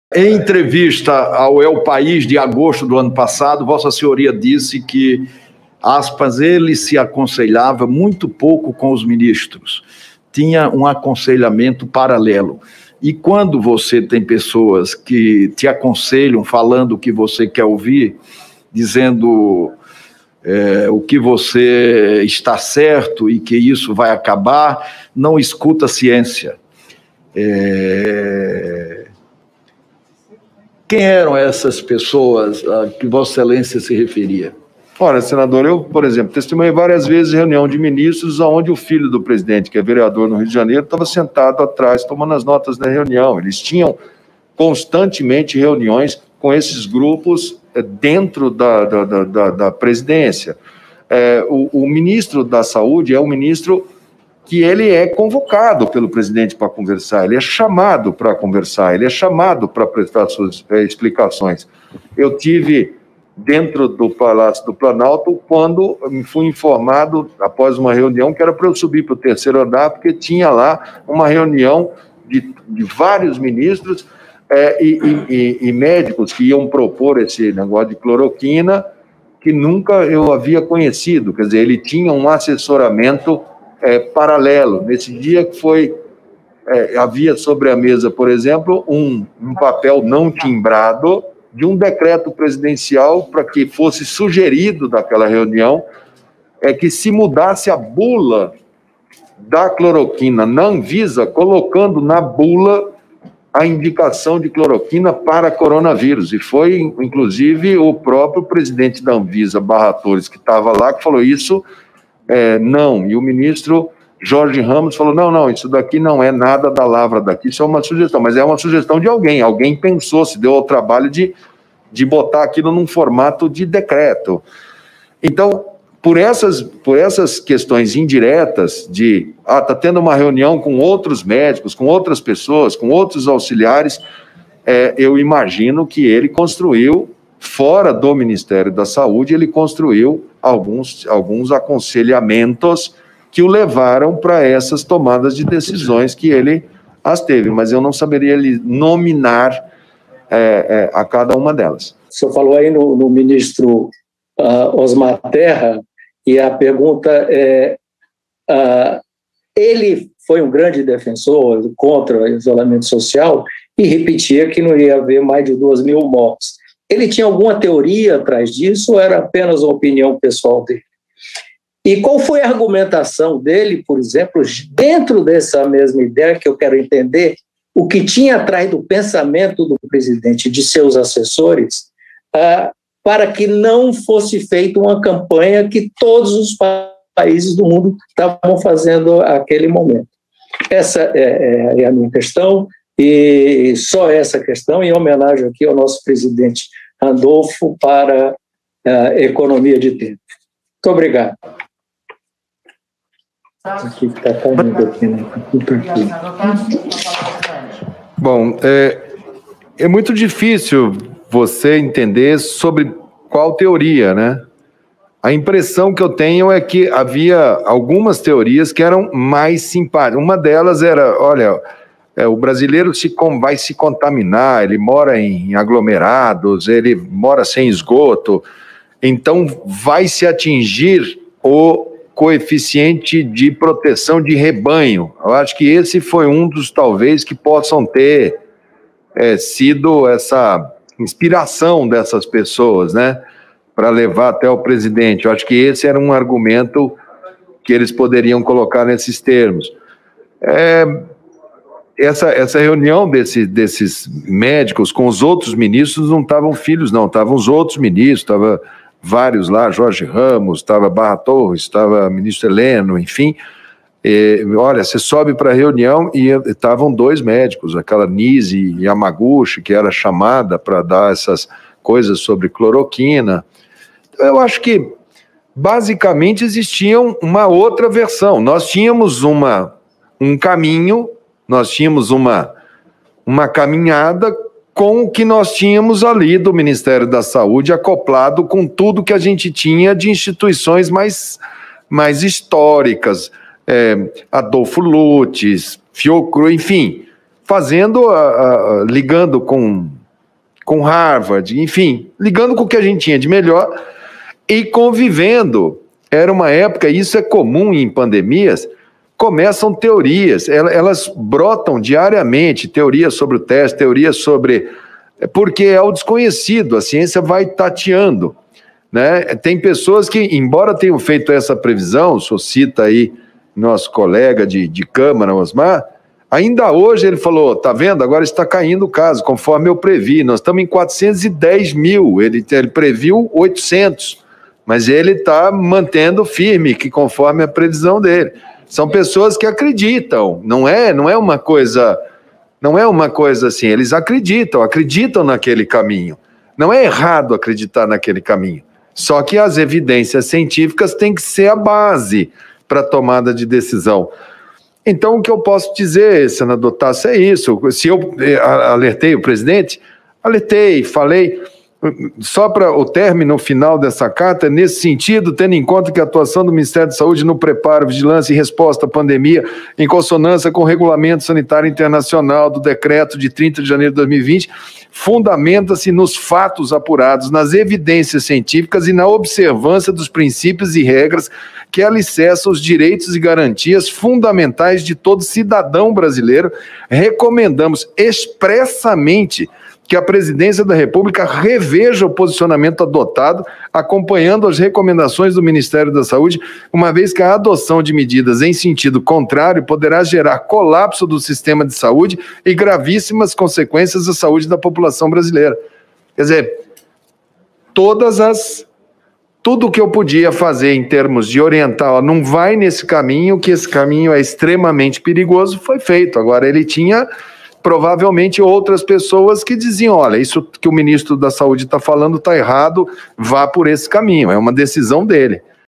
Mandetta na CPI: Carlos Bolsonaro participava de reuniões e Governo queria mudar bula da cloroquina
Em depoimento à CPI da Covid, o ex-ministro da Saúde Luiz Henrique Mandetta afirmou nesta terça-feira (4) que, durante uma reunião com ministros no Palácio do Planalto, ele teve acesso ao que seria uma proposta de decreto presidencial para fosse sugerida uma mudança na bula da cloroquina. A medida seria feita de uma forma que passasse a indicar o remédio no tratamento da covid-19.

O ex-ministro da Saúde citou ainda que testemunhou por diversas vezes a presença de Carlos Bolsonaro, vereador no Rio de Janeiro e filho do presidente, em reuniões ministeriais. "Testemunhei várias vezes reunião de ministros em que o filho do presidente que é vereador do Rio de Janeiro estava sentado atrás dele tomando notas. Eles tinham reuniões dentro da Presidência", disse Mandetta.

2- Virando membro do canal do Independente no Youtube; e/ou

O jornalismo do século XXI conta com você.